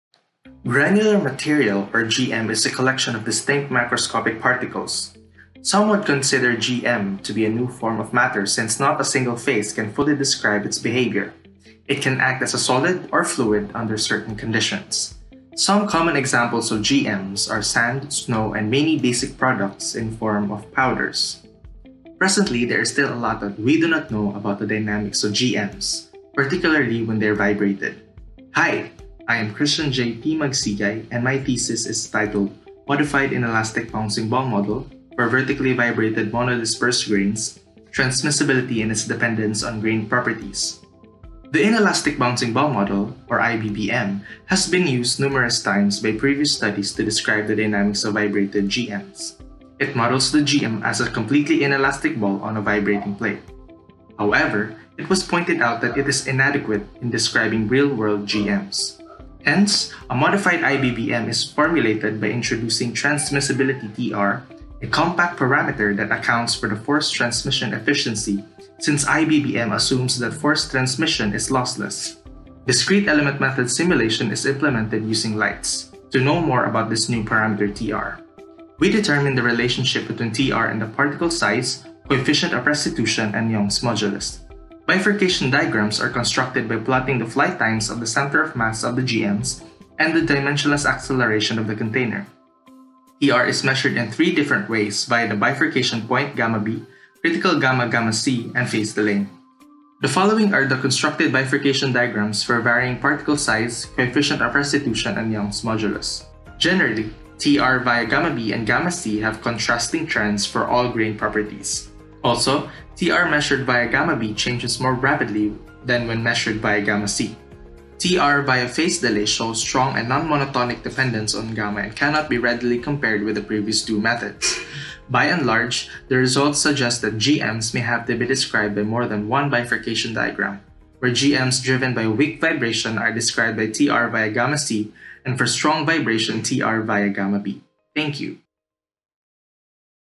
Modified inelastic bouncing ball model for vertically vibrated monodisperse grains... [CJP Magsigay]
31 May 2022 (Tuesday)
2:45 - 3:45 PM

B.S. Applied Physics Thesis Defense
Christian Jay P. Magsigay

Title: Modified inelastic bouncing ball model for vertically vibrated monodisperse grains: Transmissibility and its dependence on grain properties

Adviser: Caesar A. Saloma, Ph.D.
Co-adviser: Junius Andre F. Balista, Ph.D.
Examiners: Vince Paul Juguilon, Kimver Louie S. Nunez